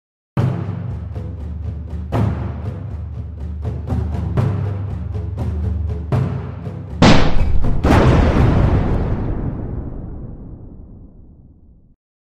Kids blow up couch in Nerf Gun War
A little Premier Pro edit of my kids pretending to blow up our couch.